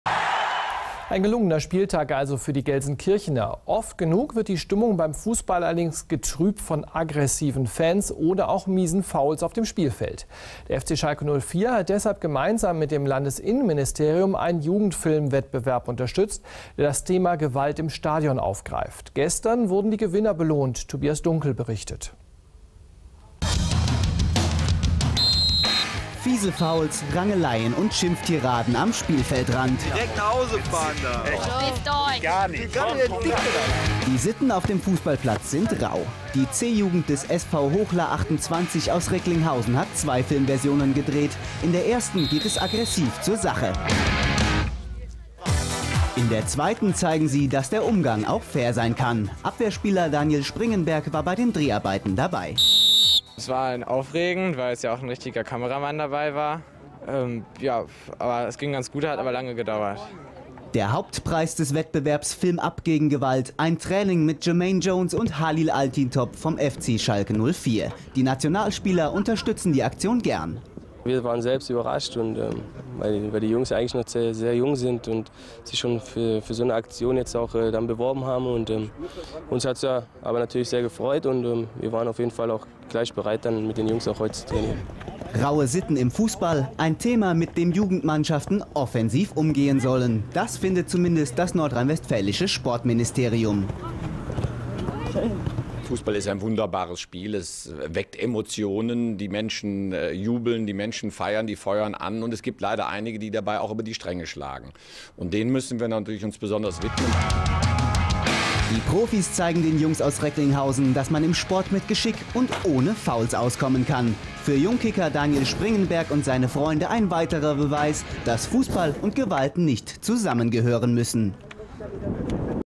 Film ab gegen Gewalt Sieger auf Schalke
Die Siegesmannschaft des SV Hochlar mit einem Training mit Jermaine Jones und Halil Altintop auf
Schalke.